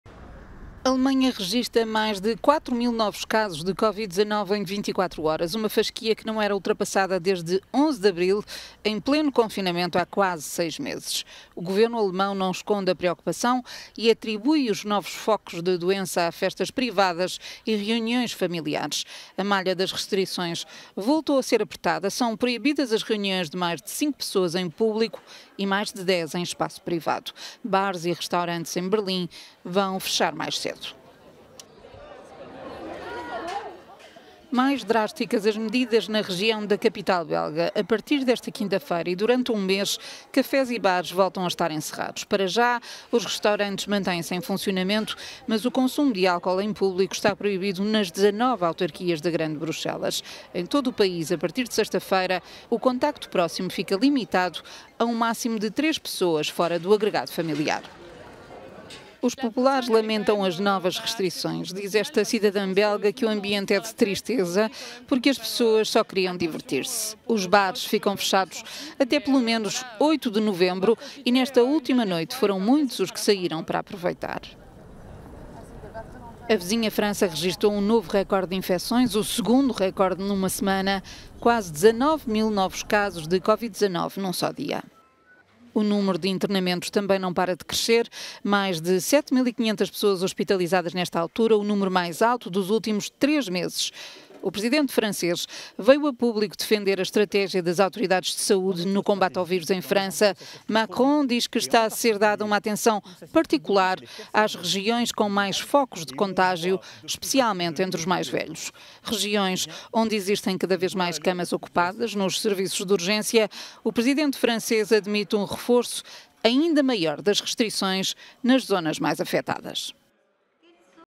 Covid-19: Alemanha regressa aos números de Abril
Por quase toda a Europa, onde aumentam os casos, aumentam as restrições